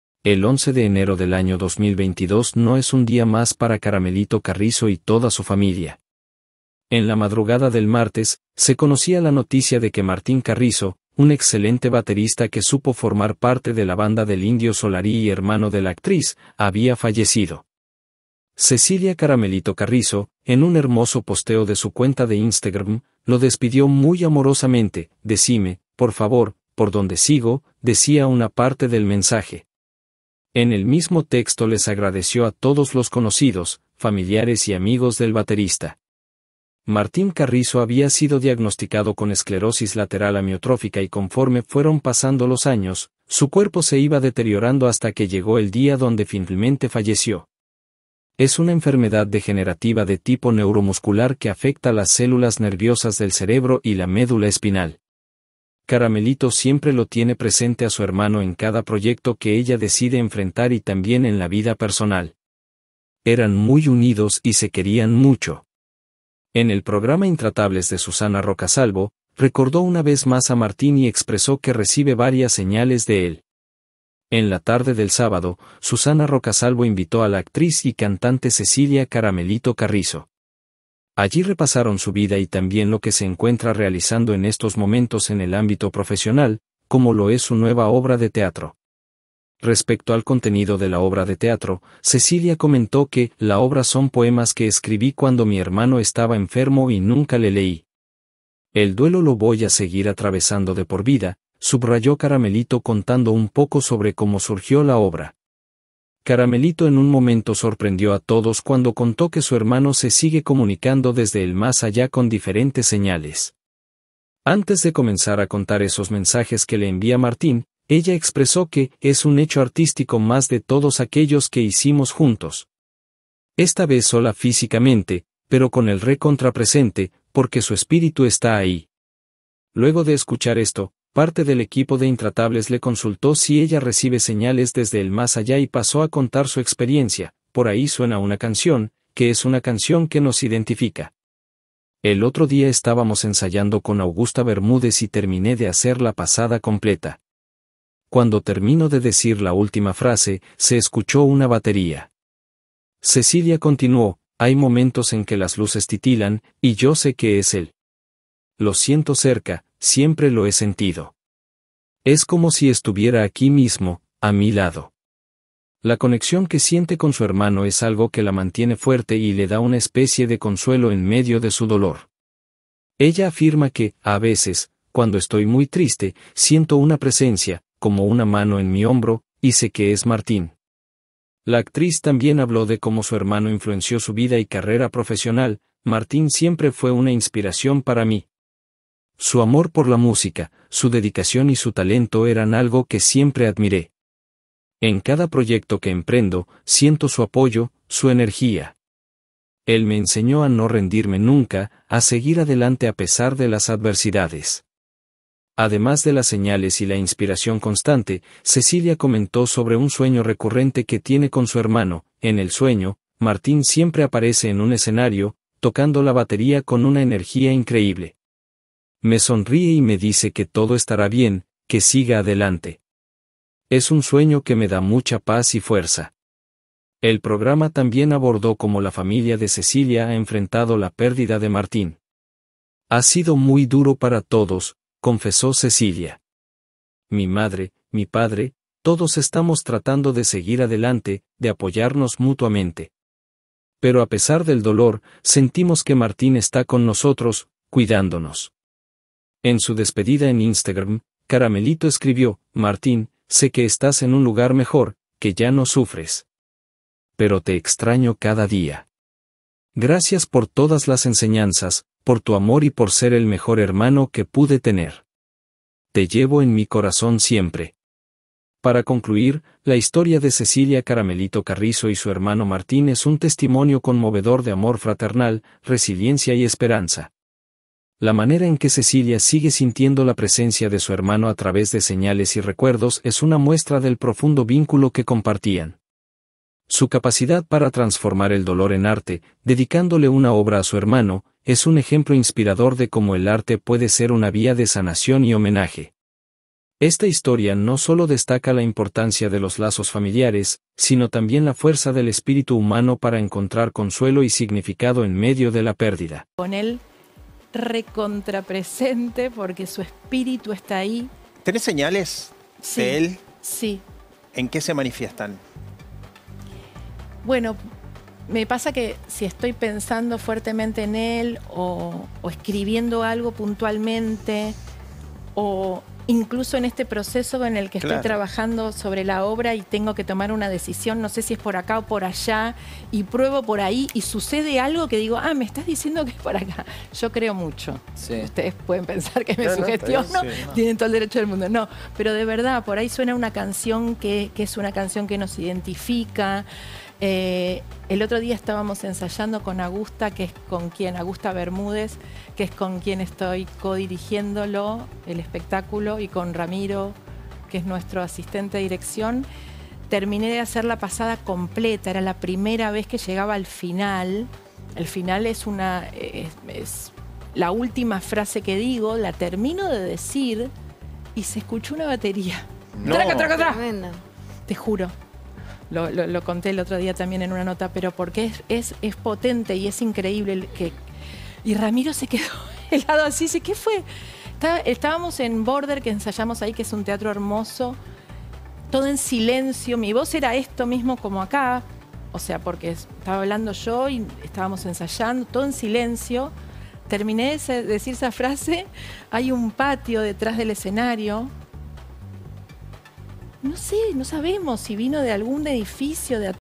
Caramelito contó cómo su hermano se comunica desde el más allá y las señales que recibe
La actriz contó en una entrevista las señales que recibe de su hermano Martín.